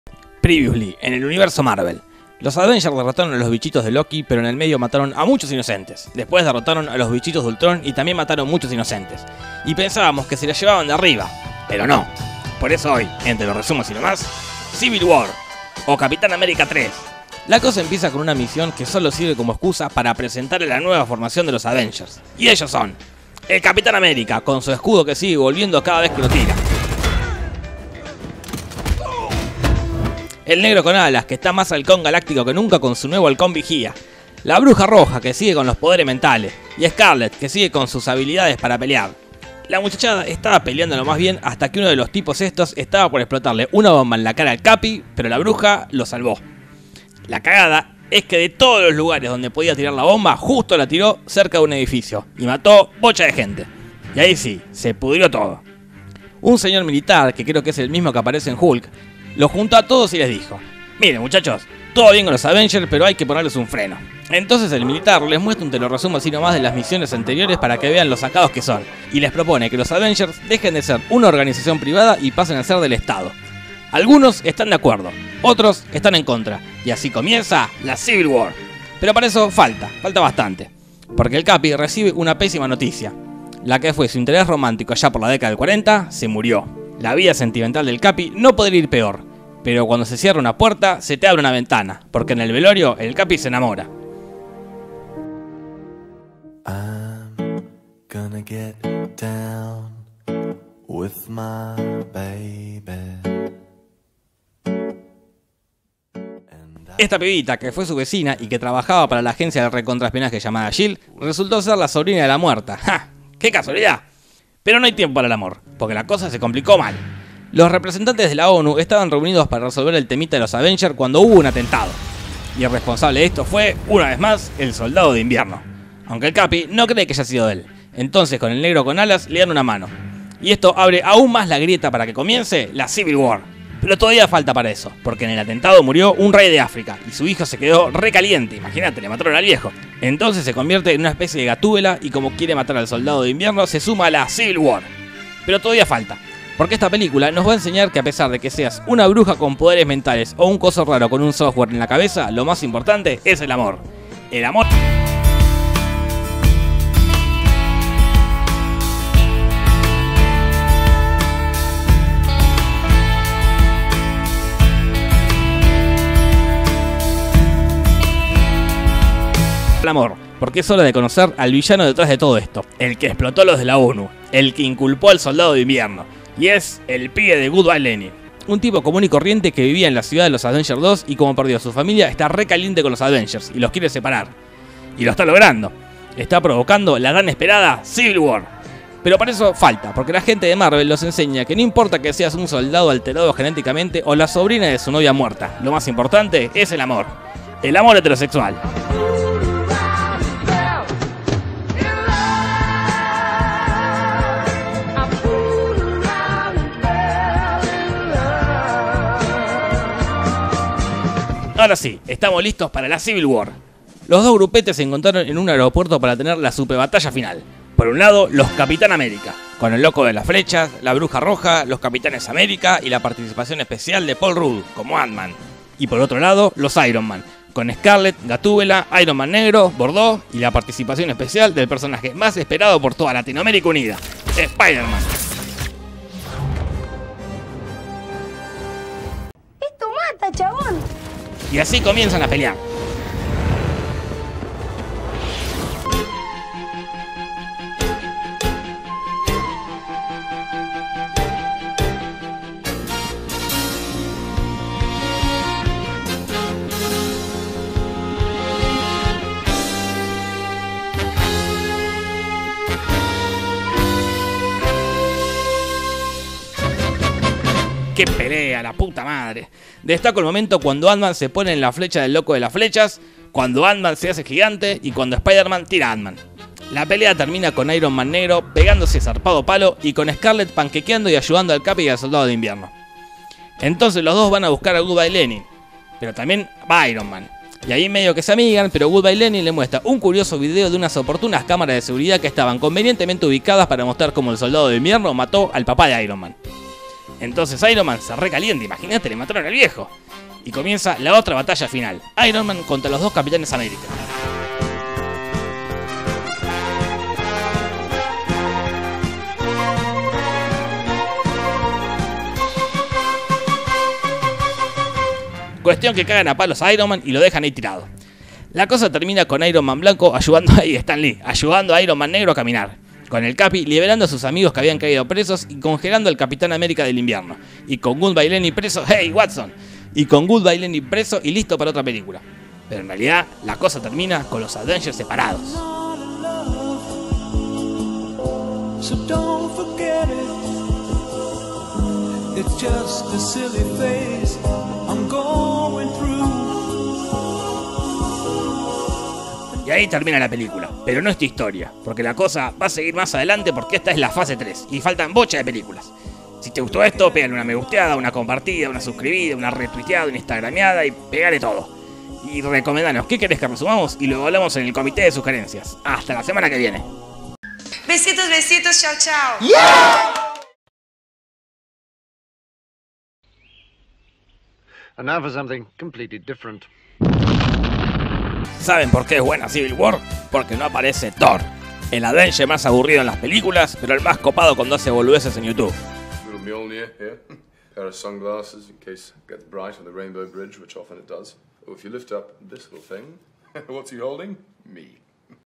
Capitan America Civil War | Te Lo Resumo Así Nomás#84
Priviusly en el universo marvel. Los Avengers derrotaron a los bichitos de Loky pero en el medio mataron a muchos inocentes. Después derrotaron a los bichitos de Ultron y también mataron muchos inocentes. Y pensábamos que se la llevaban de arriba... pero no. Por eso hoy en te lo resumo así nomás Civil War o Capitán América parte tres.

Y si te sobra el tiempo compartime el video en tu muro.